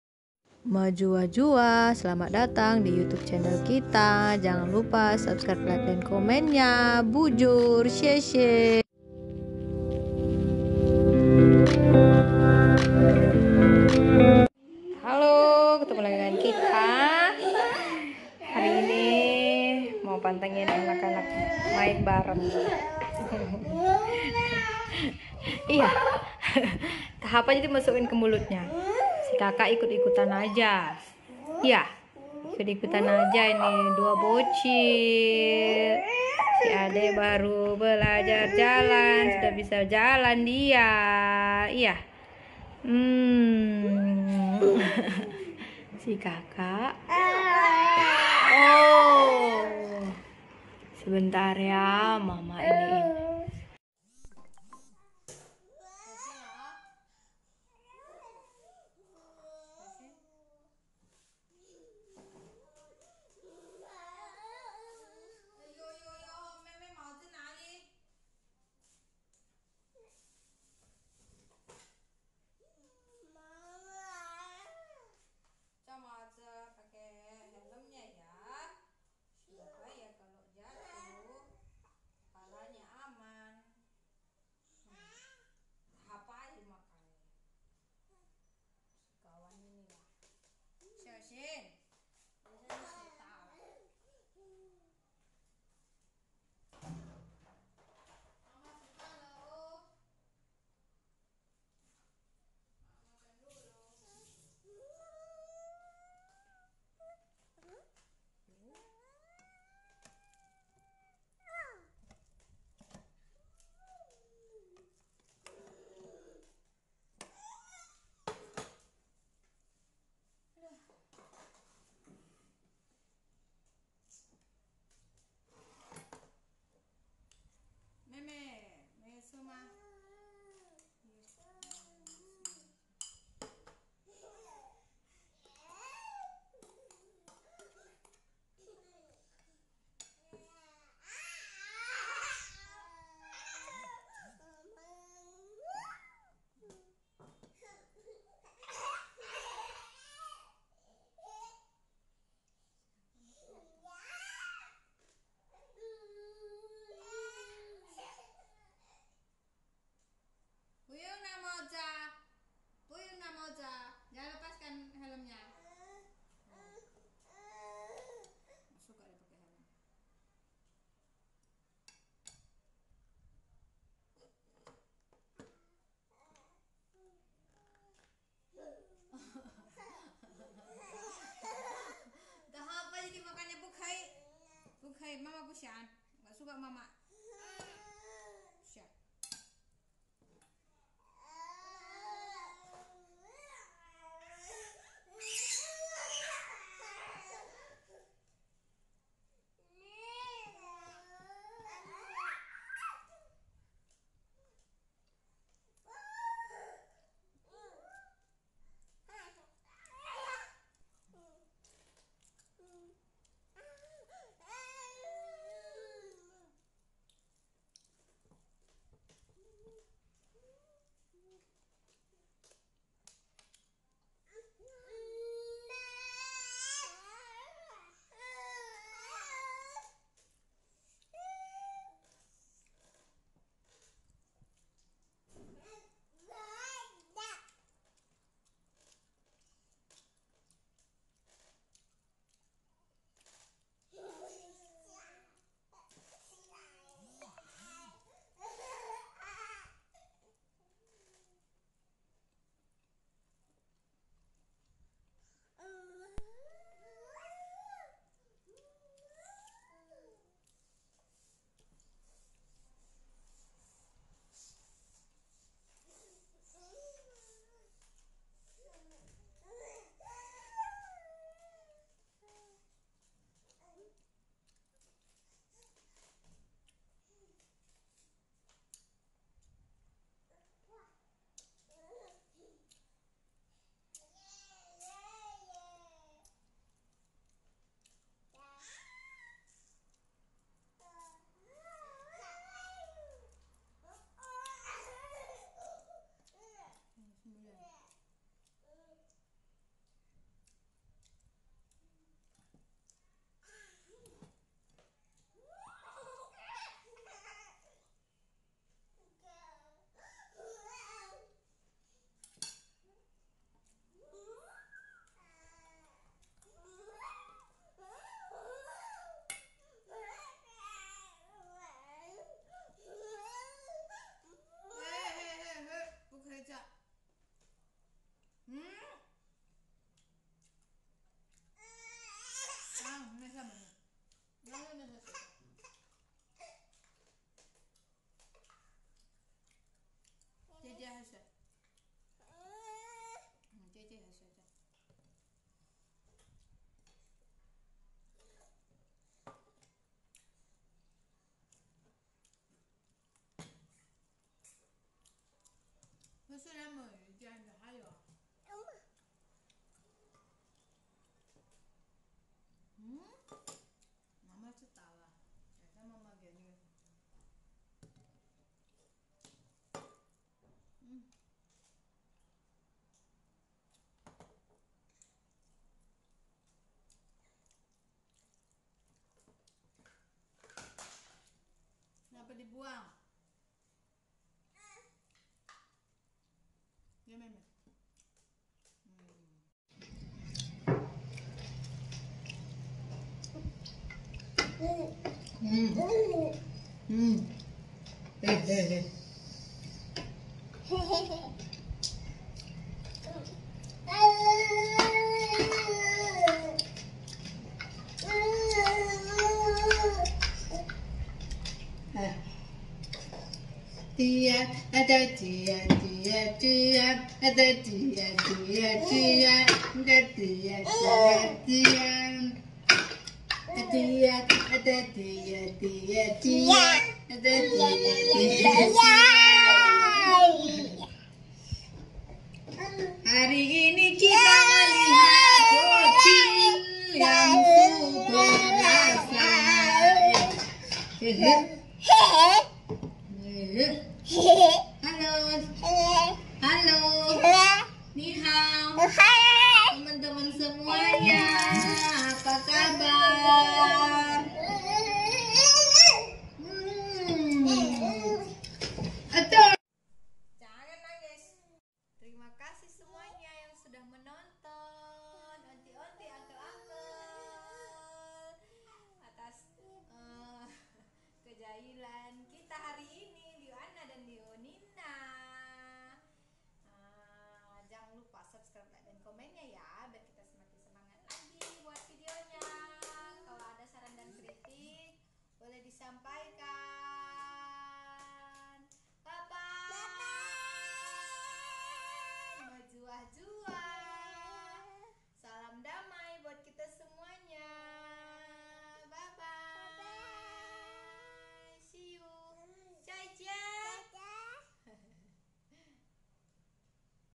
Berdua bocil tumben lagi akur main bersama bercanda bersama, kakak beradik nina anna
Hari ini mereka bermain bersama, tumben banget biasanya rebutan mainan dan si kk yang super protektif ama mainannya dan tidak mau diganggu sama sekali, tapi hari ini dia mau main bareng, entah angin apa yang melanda si kk Nina 😁😁